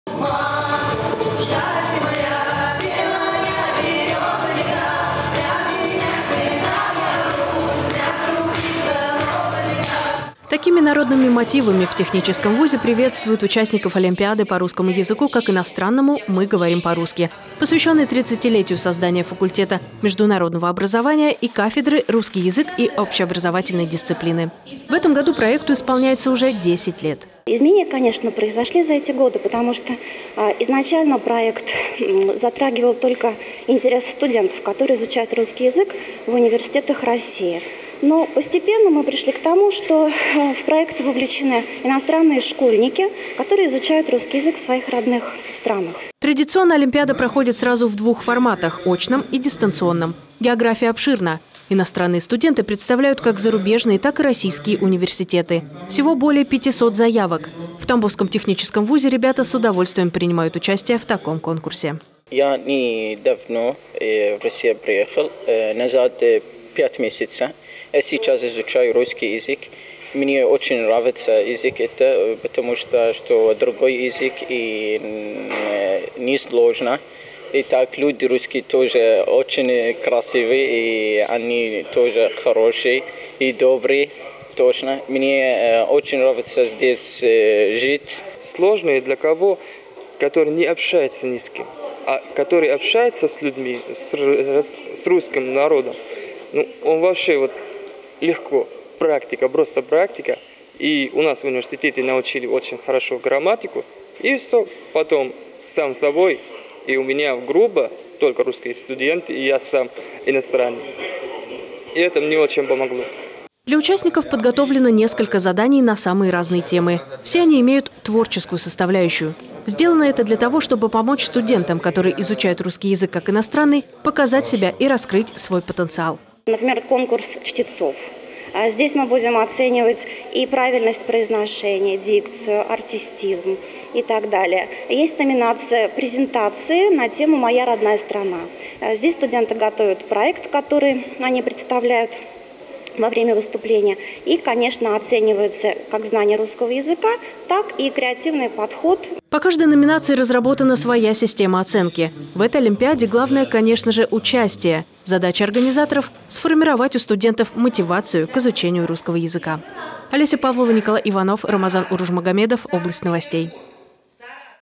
В ТГТУ для иностранных студентов прошла олимпиада «Мы говорим по-русски»
Сформировать мотивацию к изучению русского языка и раскрыть творческий потенциал. В тамбовском техническом университете уже в 10-ый раз провели международную олимпиаду по русскому языку для иностранных студентов.